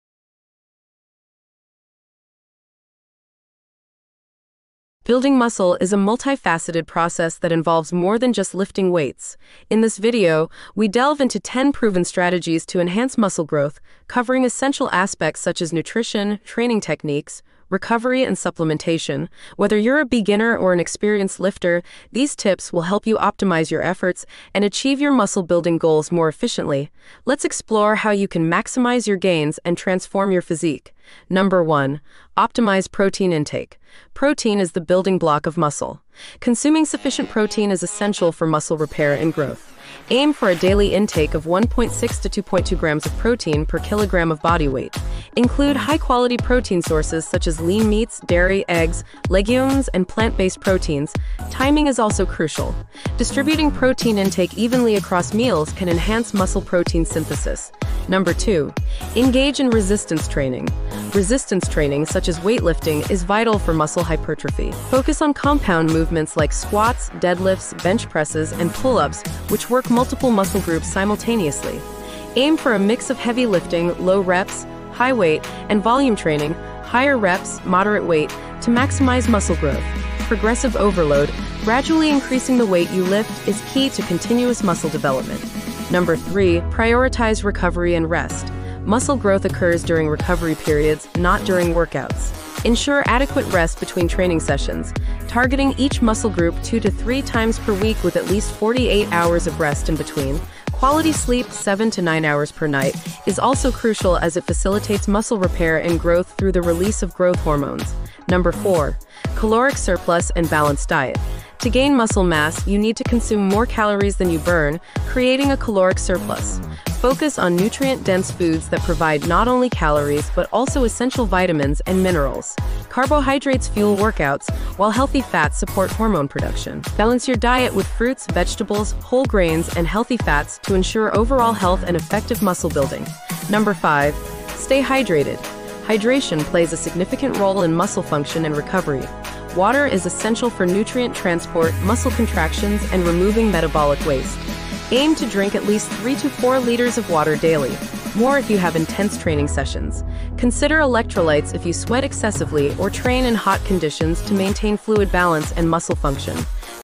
10 Proven Tips to Maximize Muscle Growth Fast Ultimate Guide for Building Muscle
Welcome to our ultimate guide on boosting muscle growth! In this video, we reveal 10 proven tips to help you maximize your muscle-building efforts and achieve impressive results faster. Whether you're new to the gym or a seasoned lifter, these strategies will enhance your workouts, optimize your nutrition, and ensure proper recovery.

In this video, you'll learn:

How to optimize protein intake for muscle growth.
The best resistance training techniques.
Importance of rest and recovery.
Creating a caloric surplus with a balanced diet.
Staying hydrated for optimal performance.
Effective muscle-building supplements.
The power of consistency and progressive overload.
Adding variability to your workouts.
The significance of proper form and technique.
Monitoring and adjusting your progress.

Watch now and start transforming your physique today!